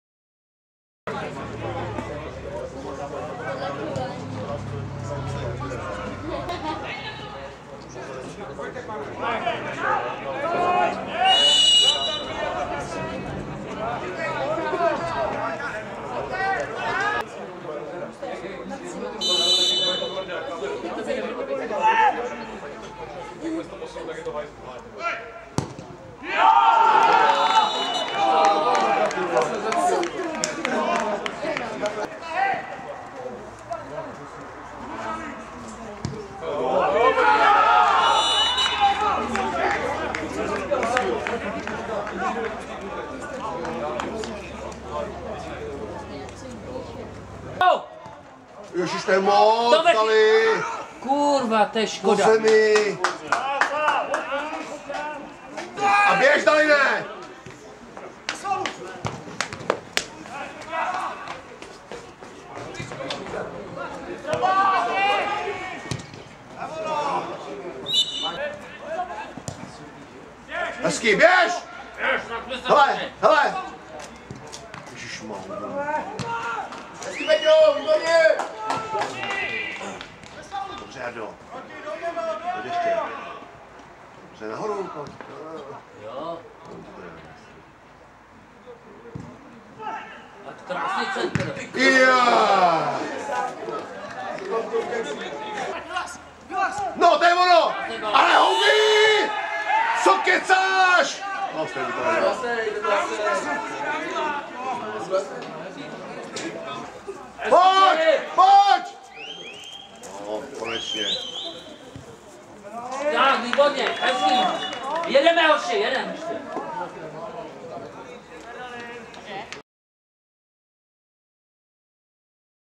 Nejdek B - Sedlec (muži) 5:3 (1.11.2015)
FK Nejdek B - KSNP Sedlec 5:3 (2:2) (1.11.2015)
Mistrovský zápas, muži, Karlovarský okres (Okresní přebor mužů).